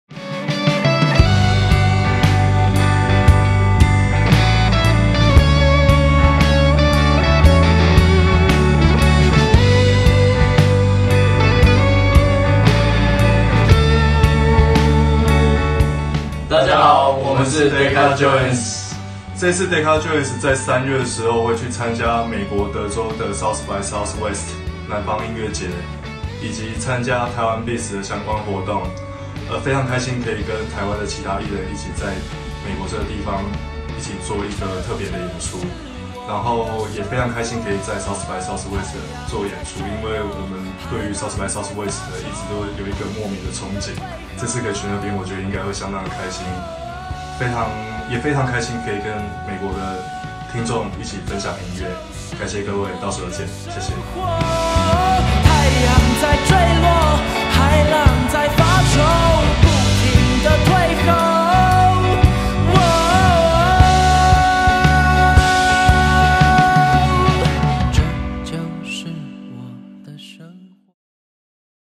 2020 SXSW x Taiwan Beats Artist【deca joins 】
台灣之夜 Taiwan Showcase

頹廢的浪漫情調《deca joins》
隨著旋律叫醒躊躇的慢靈魂

03/19
SXSW Taiwan Showcase
@ Elysium Austin
Door Open：7pm